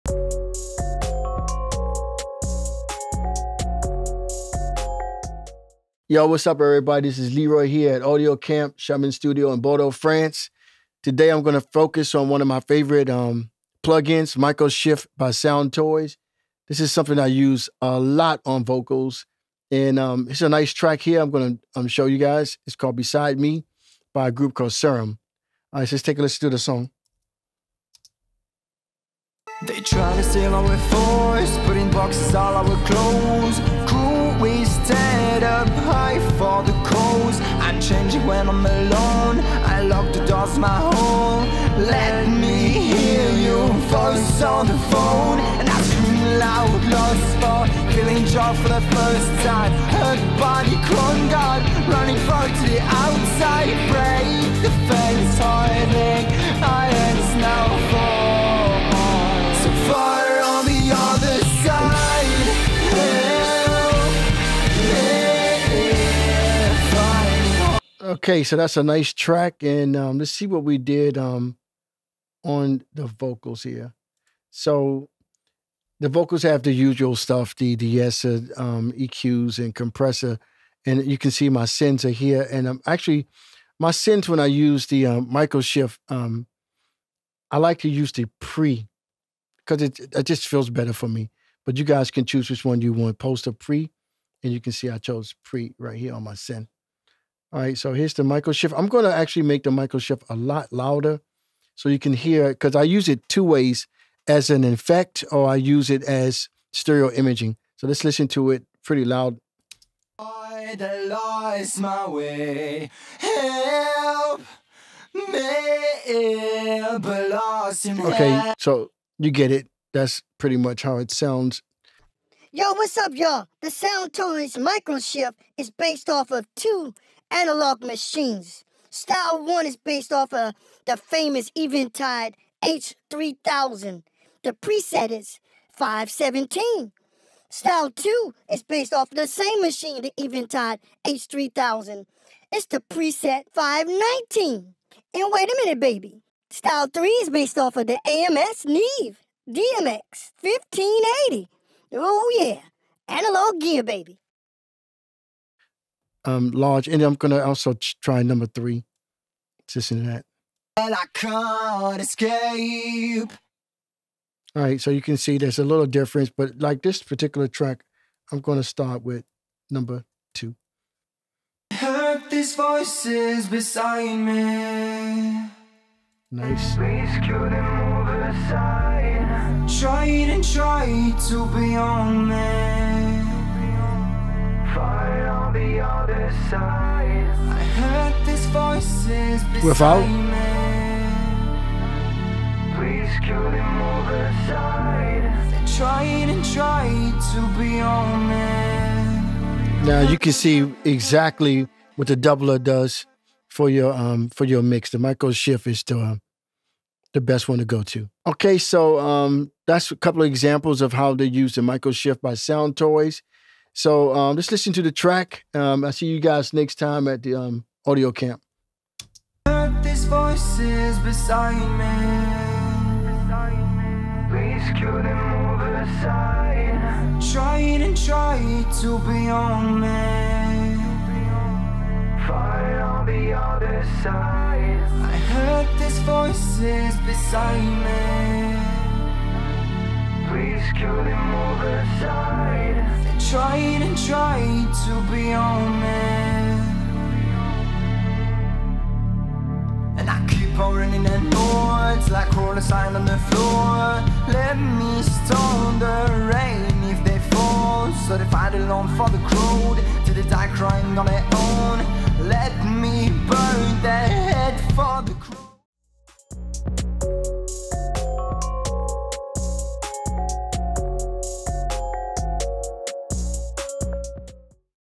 SoundToys Microshift Tutorial I Plugins Masters
@leroychambers_ from PluginsMasters is introducing Microshift by @Soundtoys